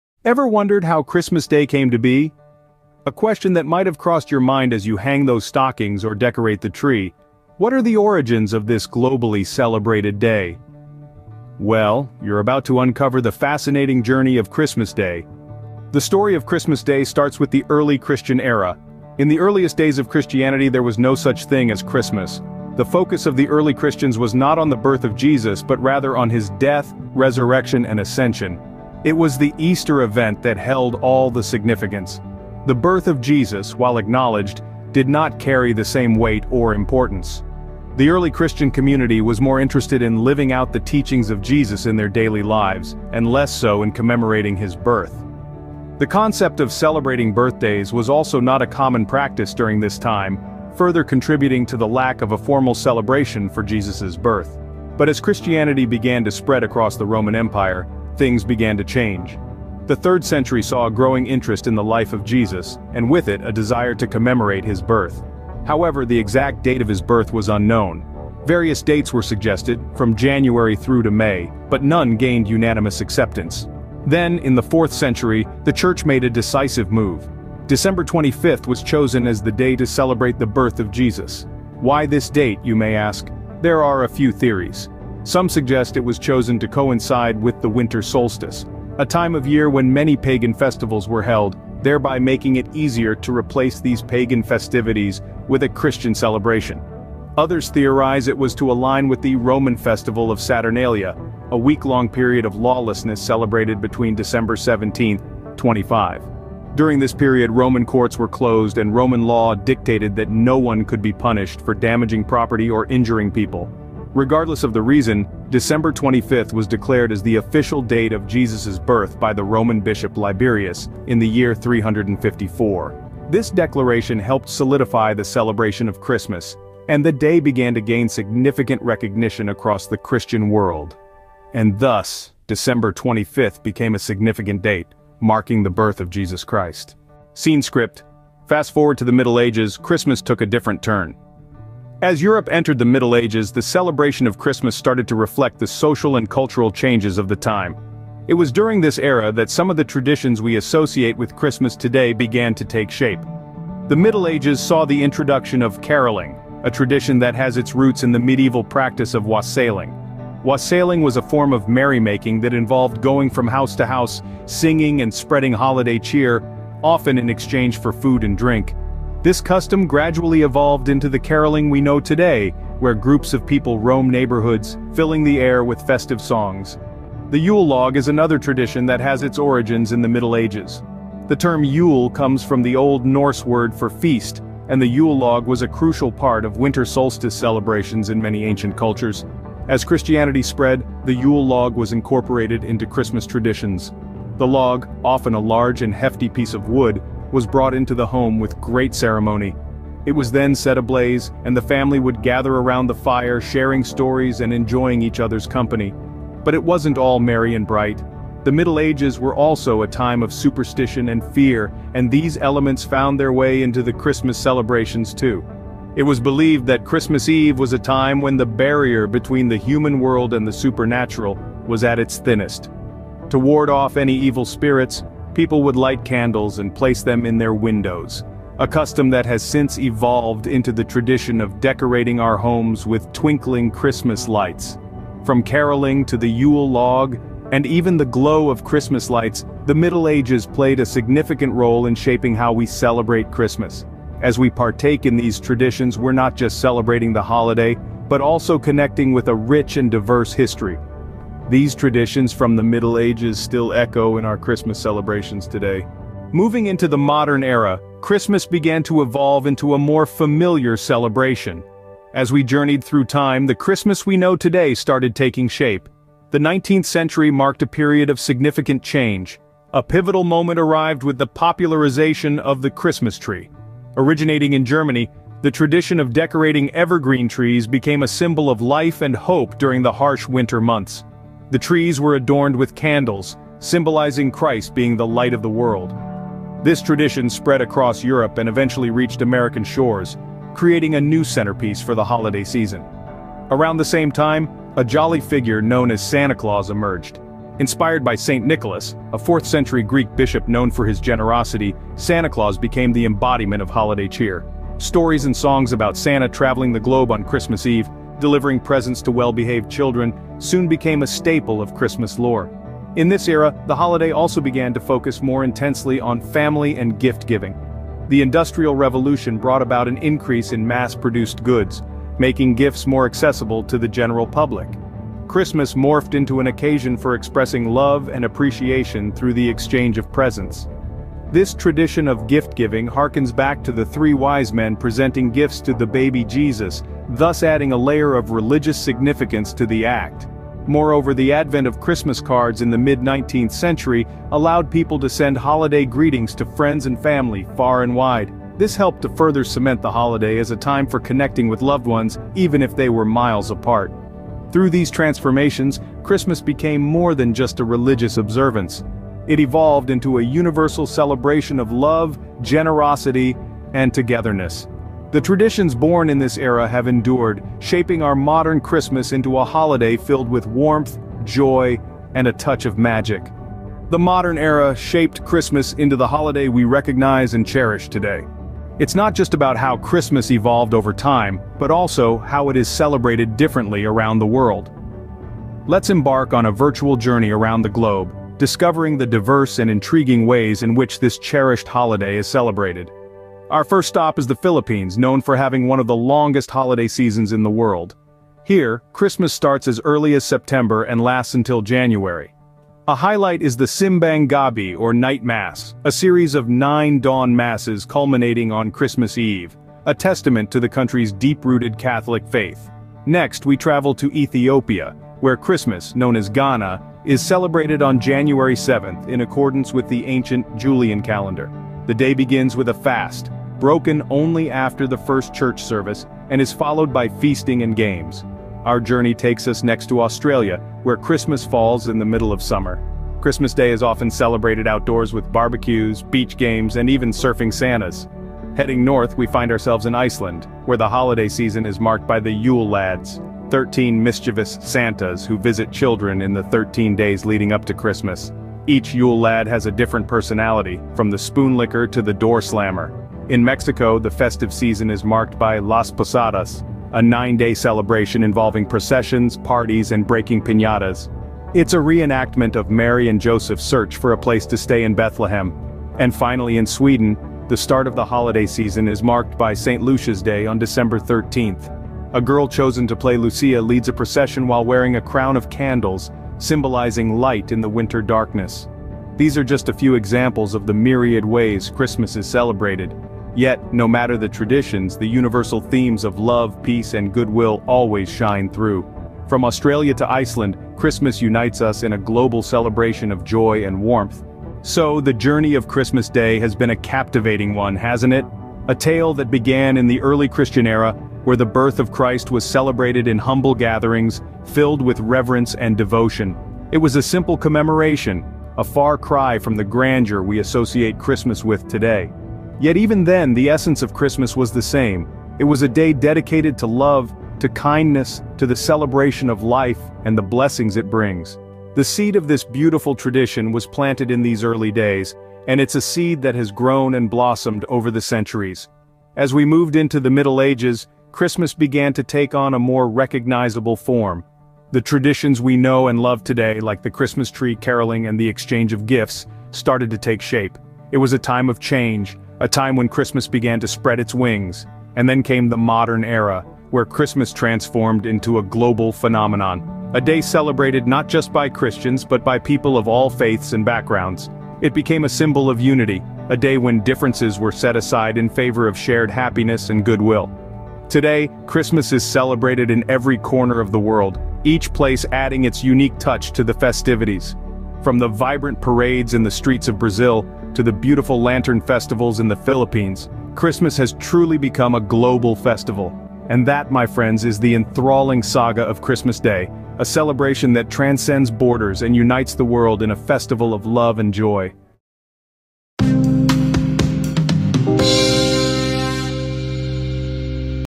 Unwrapping The Story of Christmas Day
In this captivating video, we dive deep into the enchanting story of Christmas Day and uncover its rich and fascinating history. Join us as we explore the origins of this beloved holiday, from ancient traditions to modern customs. Learn about the historical figures, rituals, and myths that shaped the celebration of Christmas as we know it today. From the birth of Jesus to the influence of pagan traditions, this video paints a compelling portrait of the holiday's evolution. Come along on this festive journey and gain a deeper appreciation for the traditions that bring us joy each December 25th.

OUTLINE:

00:00:00 An Intriguing Question
00:00:15 Birth of Christmas – Early Christian Era
00:02:17 Christmas in the Middle Ages
00:04:16 Christmas in the Modern Era
00:06:18 Christmas Around the World
00:08:14 Reflection on the Journey of Christmas Day